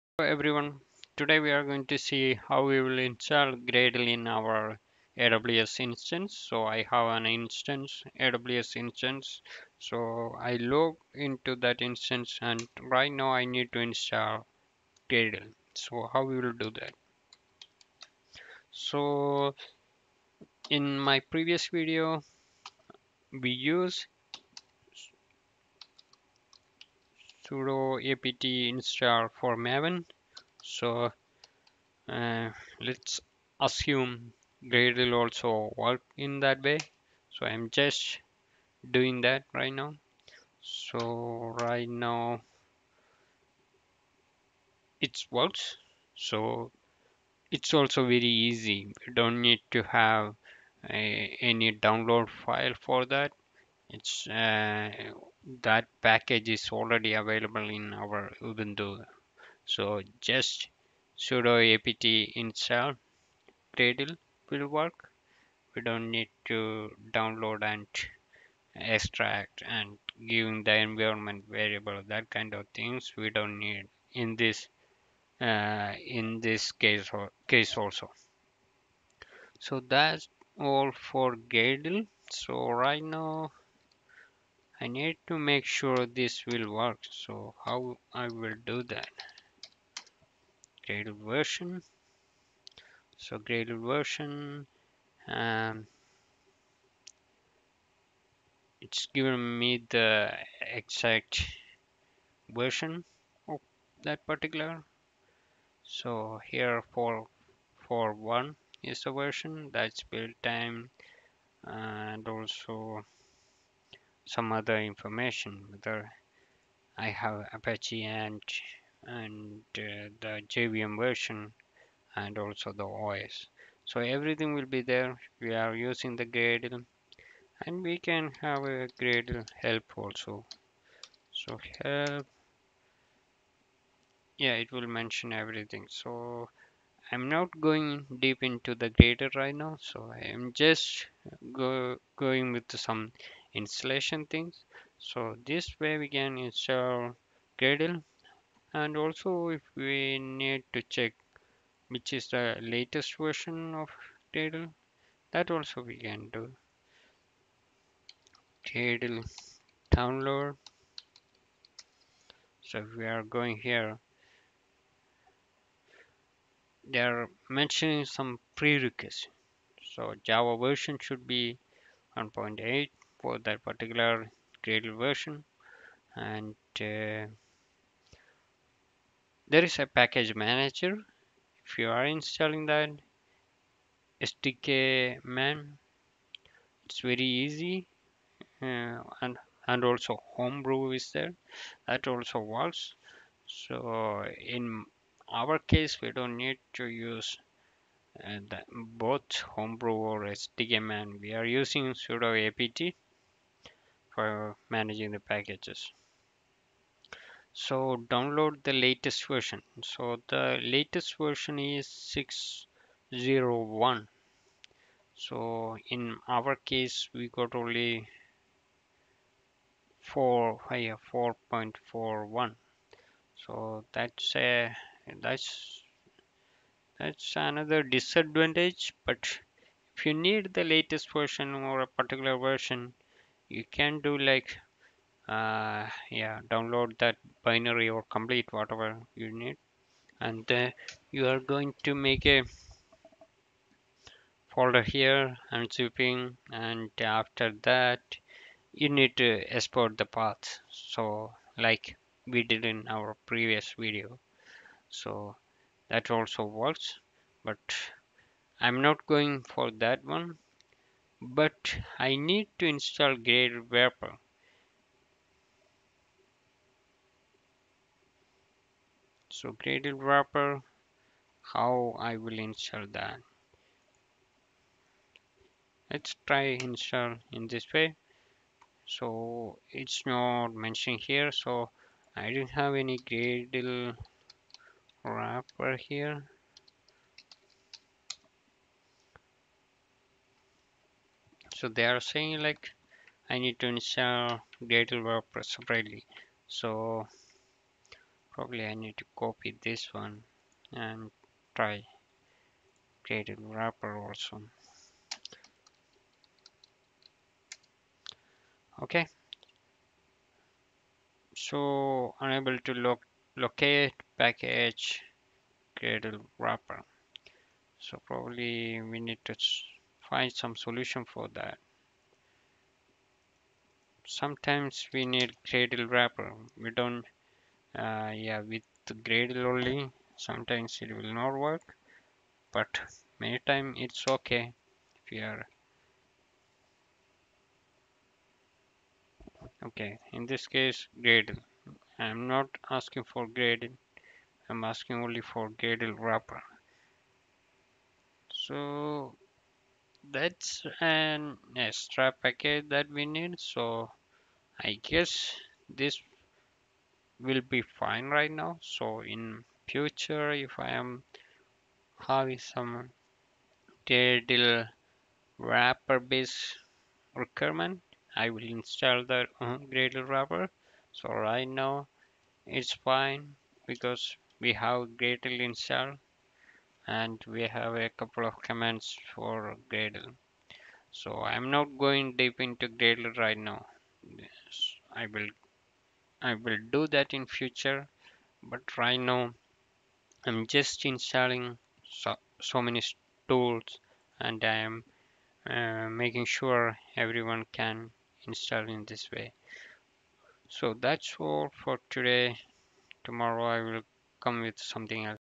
Installing Gradle in AWS EC2 ubuntu
log into AWS console
use sudo apt install gradle
try with gradle --version